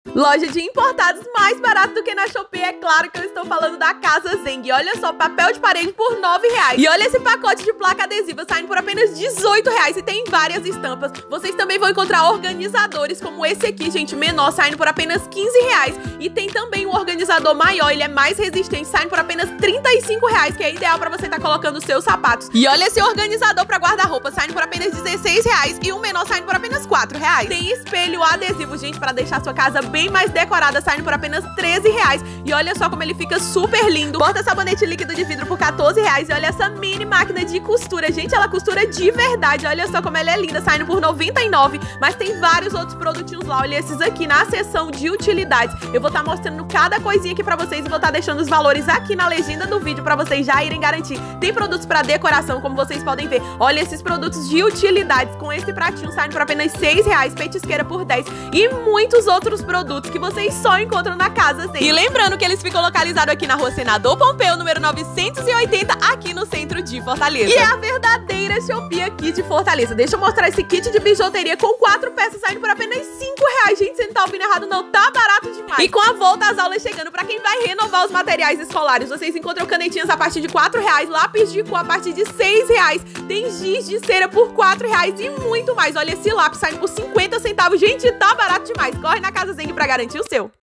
E vem conferir as novidades dessa loja de importados que tem produtos mais barato que a shopee.
E vem conferir as novidades dessa loja de importados que a shopee de Fortaleza!

Loja @casazheng_fortaleza

Localizada Rua Senador Pompeu 980, Centro